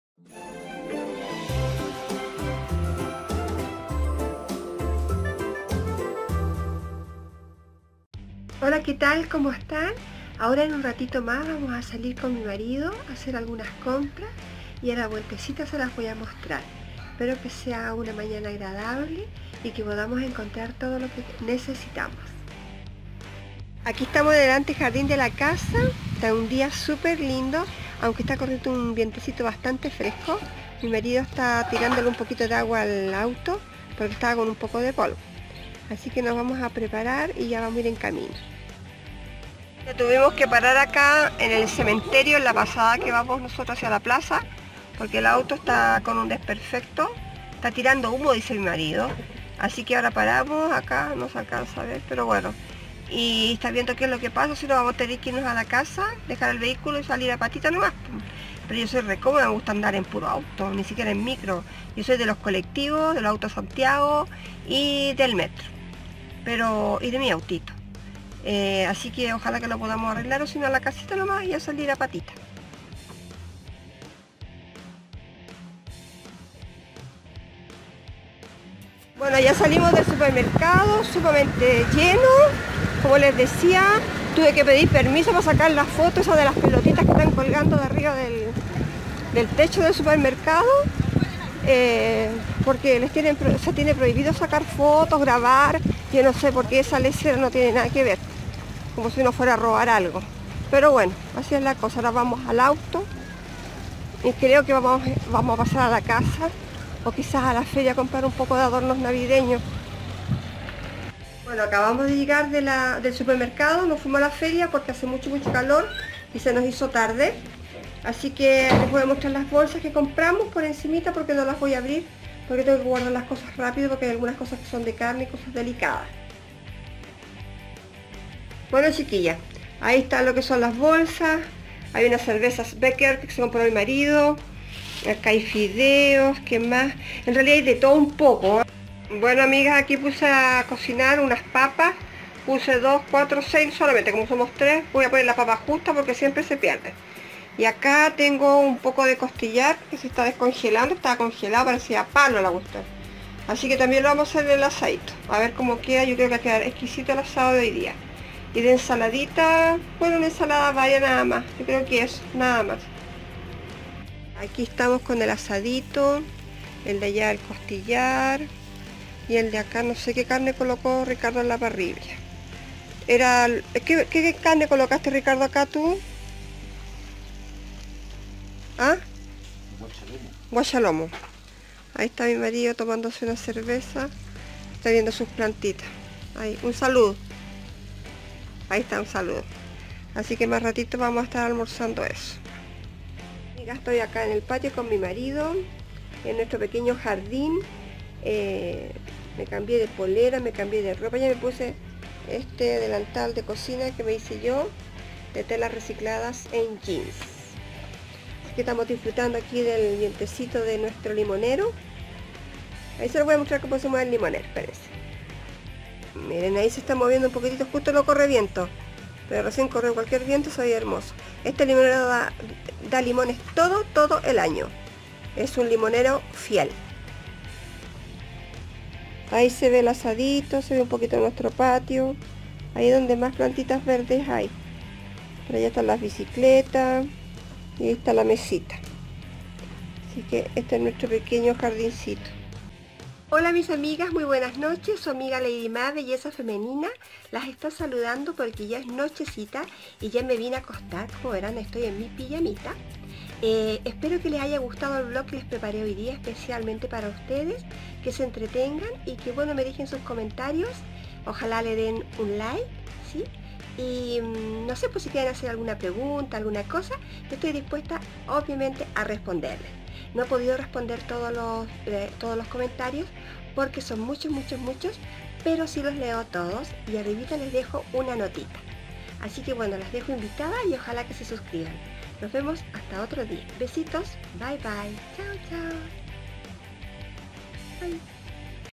VLOG # 22 /BUSCANDO ADORNOS NAVIDEÑOS/ SUPERMERCADOS /ASADO FAMILIAR
Sera un dia de entretencion para ustedes terminando con un rico asado familiar